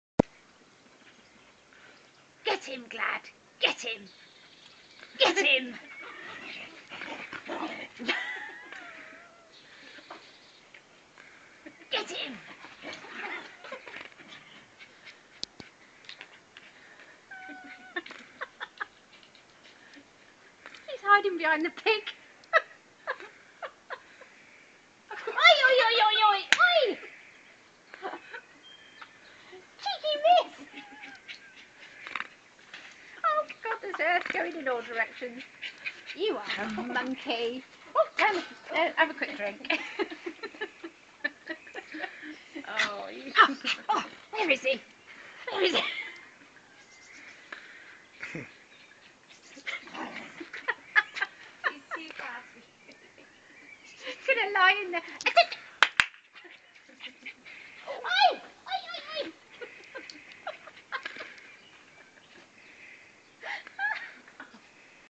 Gladys the Dog playing with her very best friend.....Billy !
Gladys the oft spoken about Dog (she's the small Border Terrier) playing with her very best friend. Billy in my back yard !